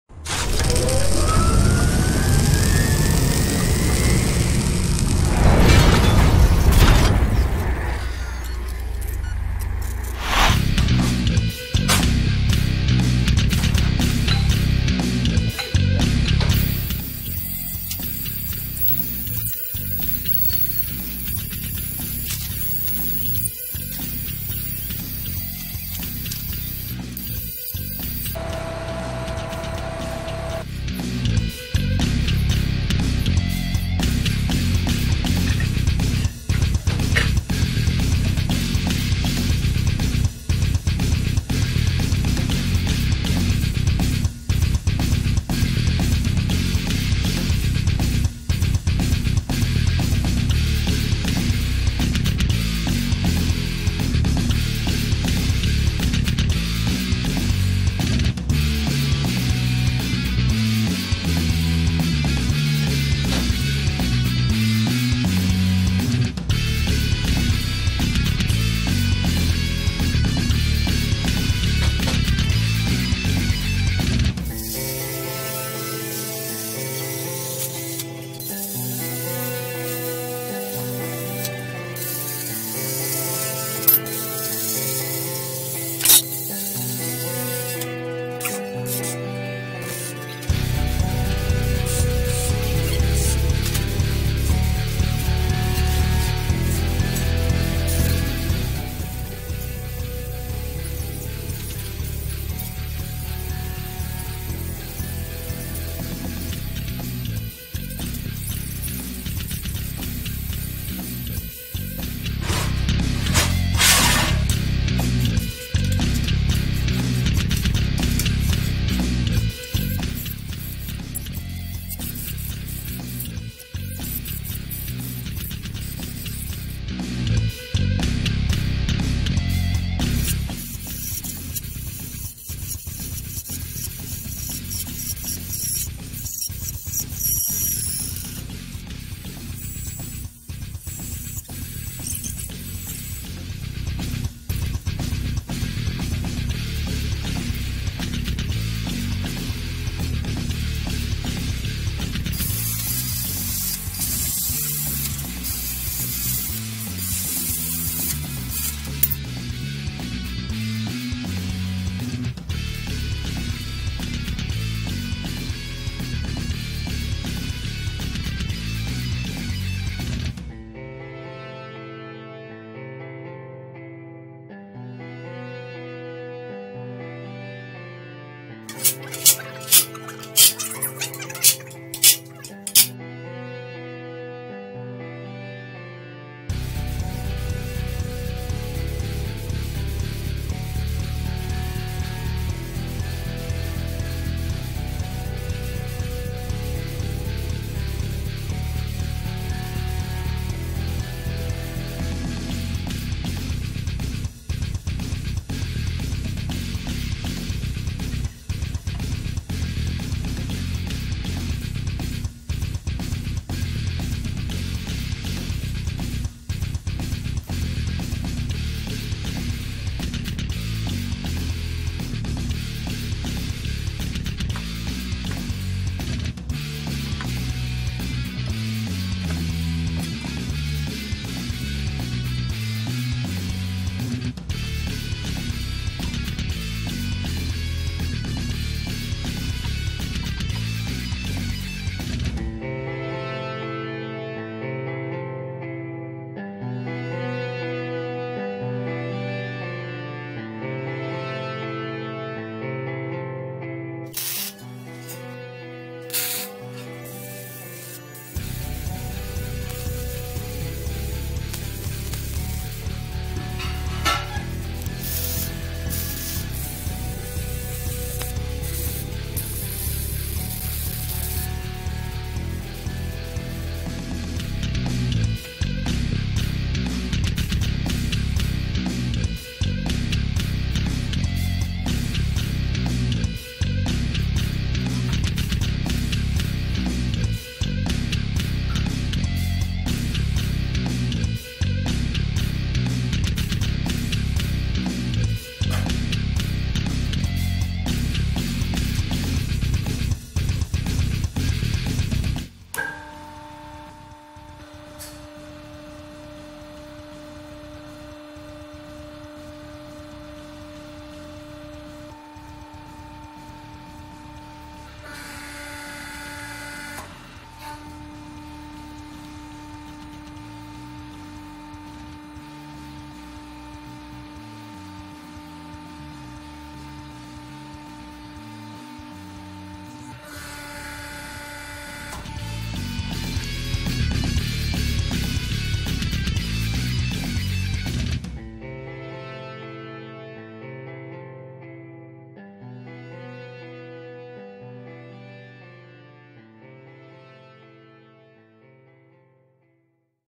Project 068 | Hydraulic Power Pack
After making a plunger presented on one of our previous videos and as we make machines that work using plungers, we considered it necessary to add a mobile, hydraulic unit in our shop too.

►Tools in this video:
Engineers Squares Set
25FT Tape Measure

• Special Thanks to our Patrons:
✔TOP Patrons: Jesse Burton, Devin Bauste, Jeff Wall